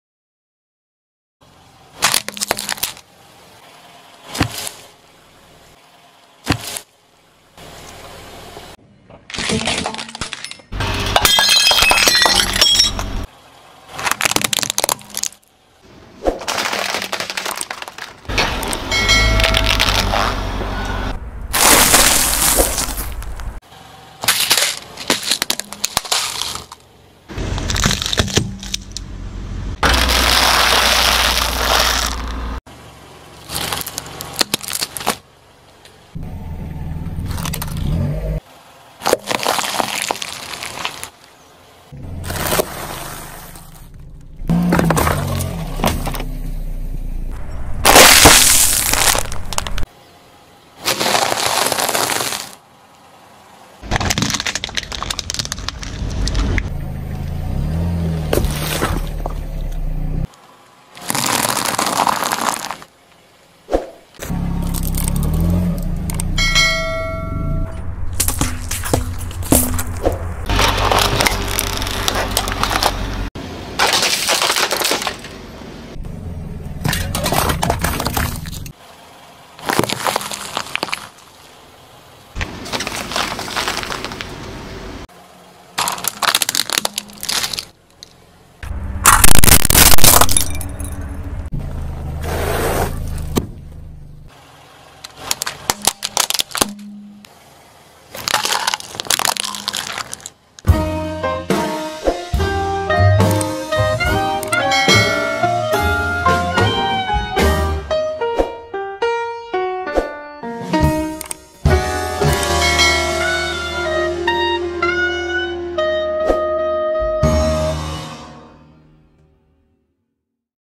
Experiment-56: CAR VS WATERMELON | Crushing Crunchy & Soft Things by Car! | ASMR
Relaxing ASMR
Satisfying Video